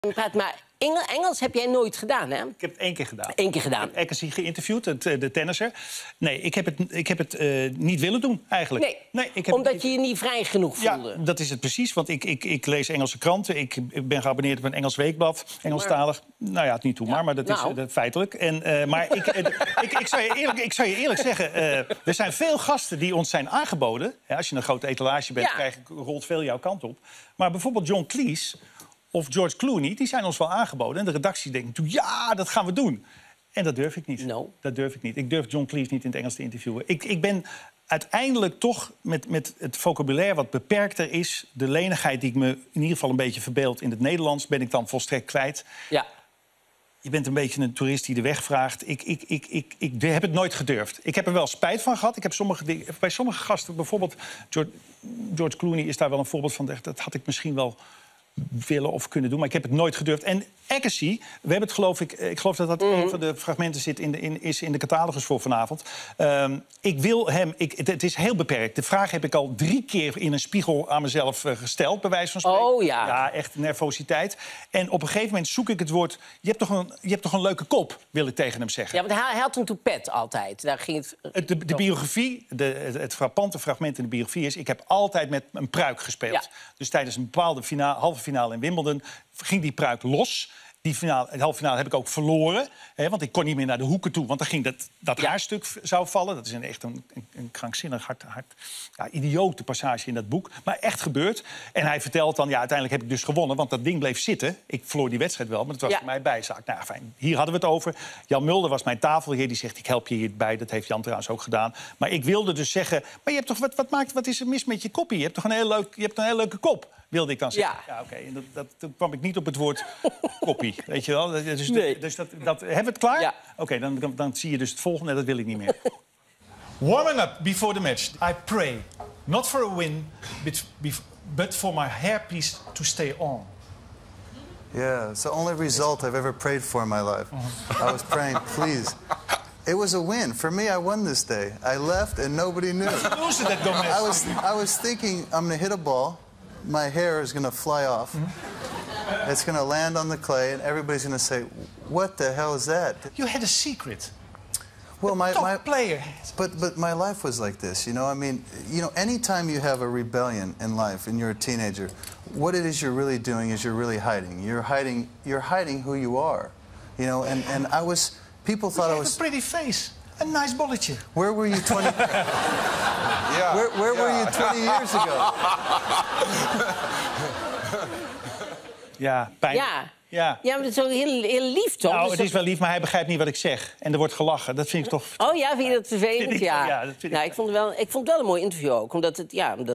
Matthijs wilde niet in het Engels interviewen
Interviewen in het Engels deed Matthijs liever niet. Zelfs John Cleese en George Clooney konden hem niet van gedachten veranderen. Hij deed het één keer, met tennisser Andre Agassi.